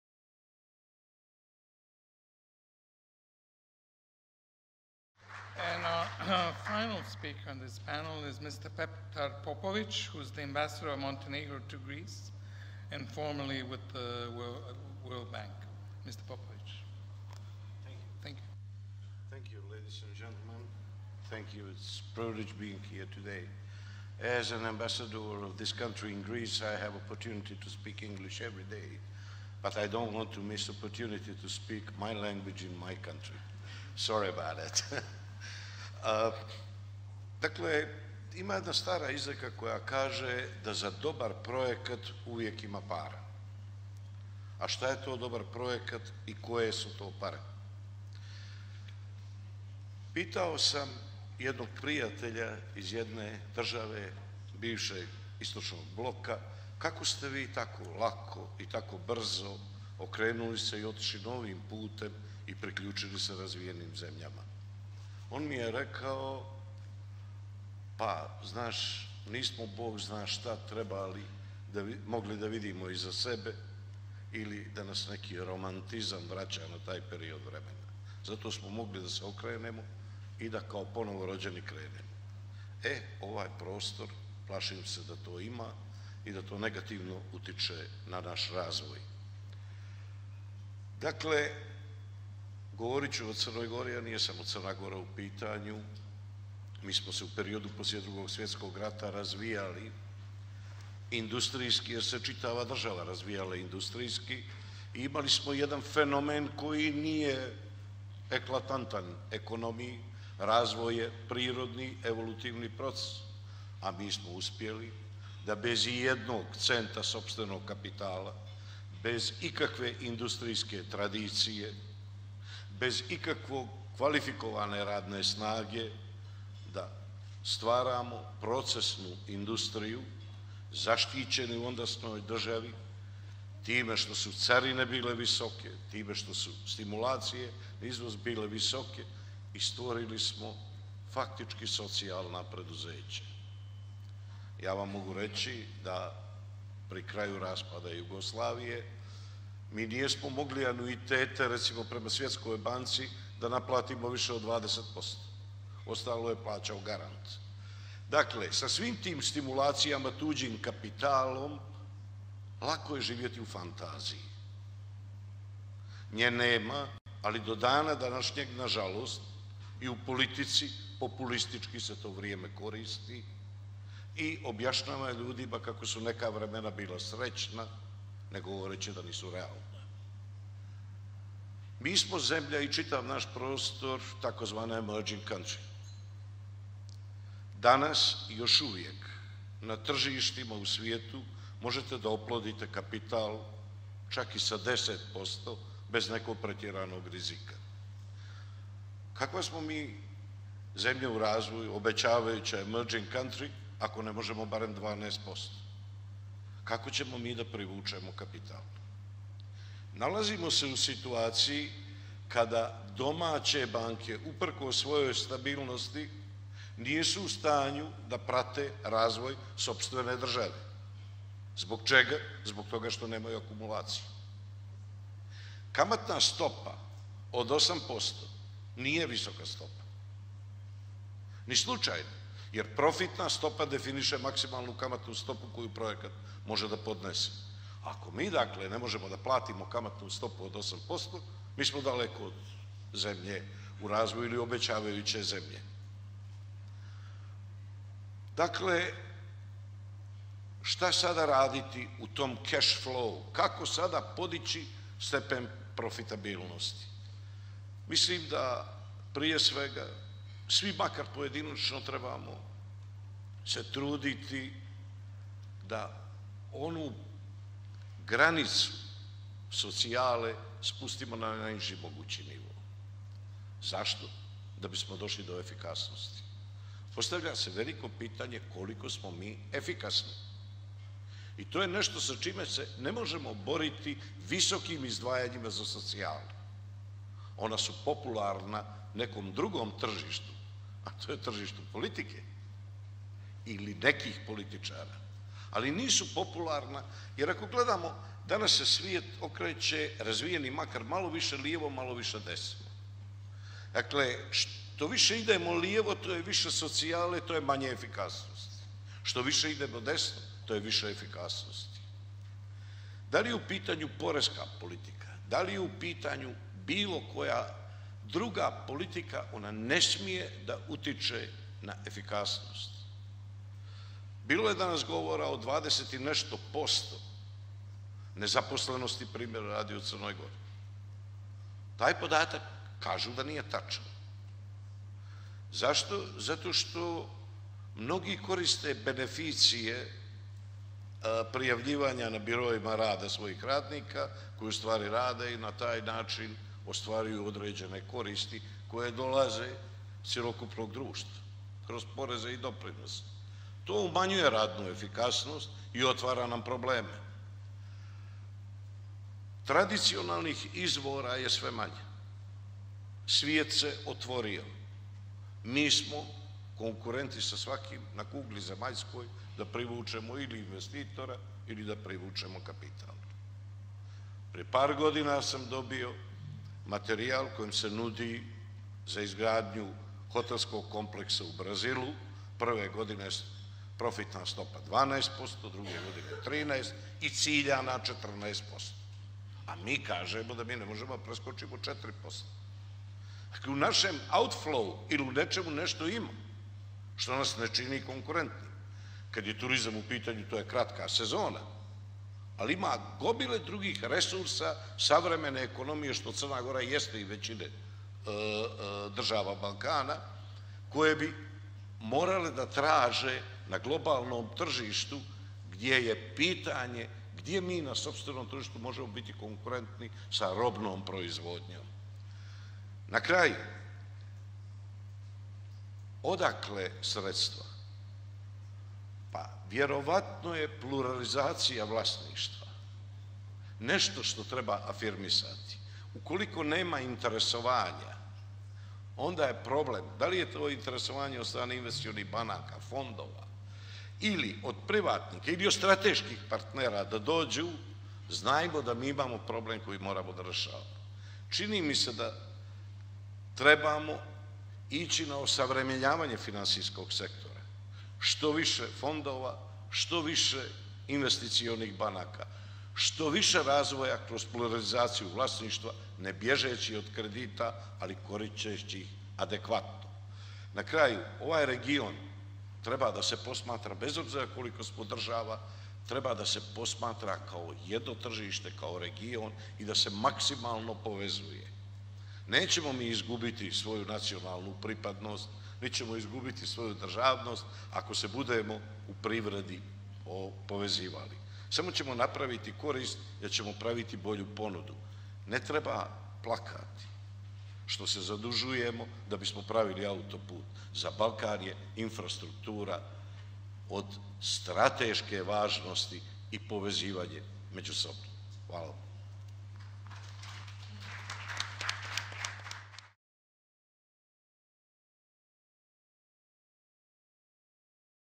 Petar Popović, ambassador of Montenegro to Greece
Petar Popović at The Economist's Sixth EU-SE Europe Summit in Montenegro, May 3rd 2017